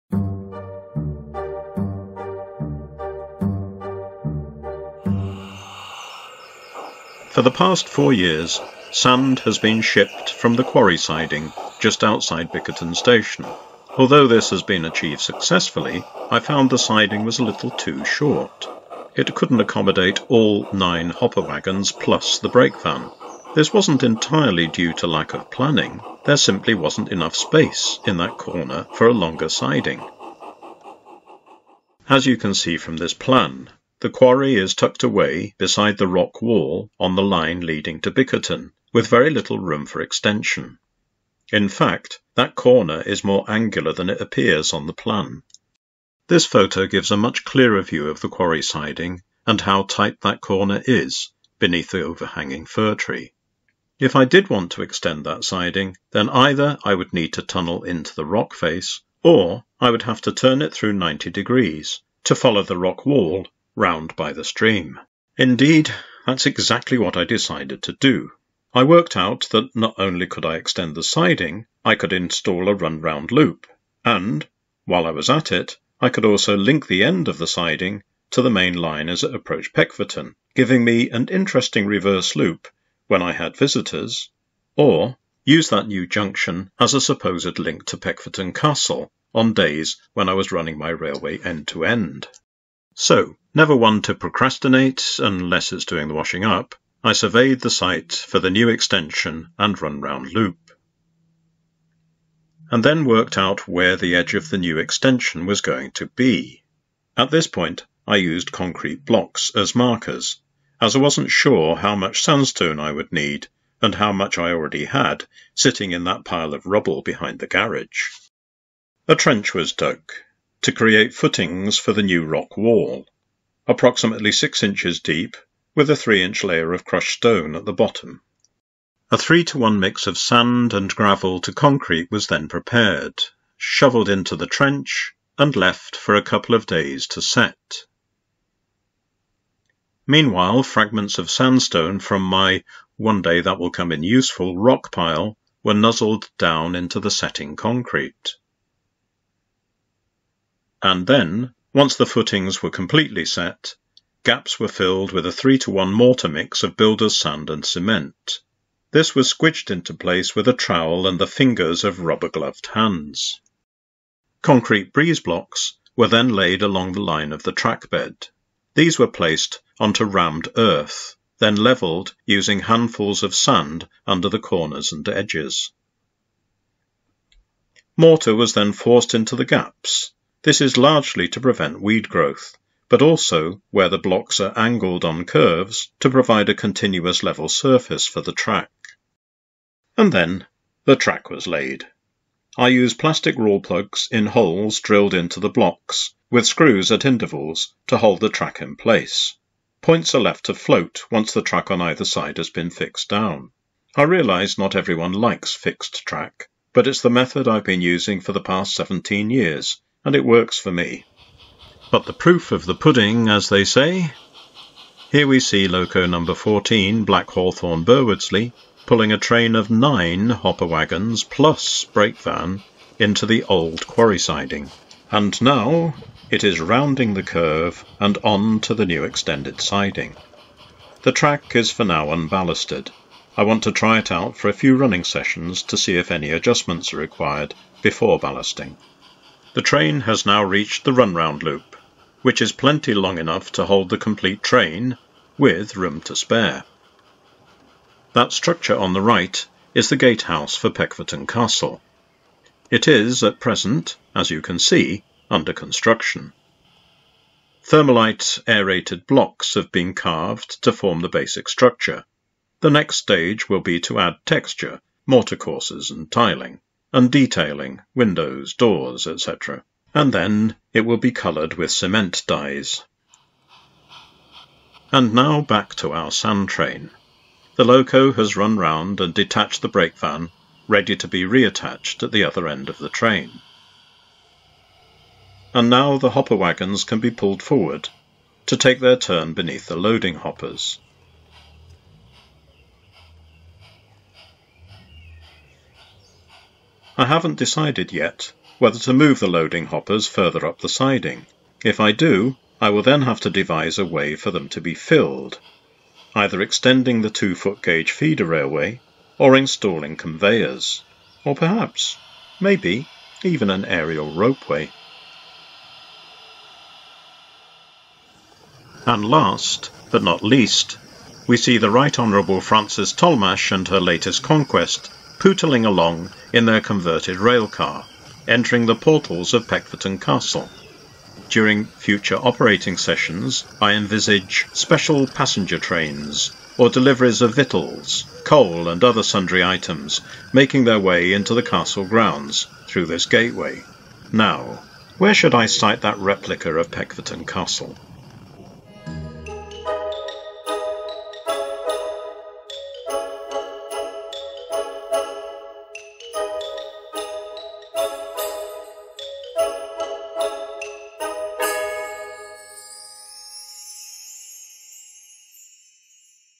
Peckforton Light Railway - The Sand Quarry Extension
I have now completed the majority of work on the sand quarry extension. The original siding was too short because of lack of space. Rather than adding just a few centimetres to the siding, I decided to extend it further to include a run round loop and link it back to the main line. Peckforton Light Railway is a mythical three foot narrow gauge railway which might have served the copper mines and local communities beside the Peckforton Hills in Cheshire UK. It is modelled in 15mm scale and 45mm gauge in the garden.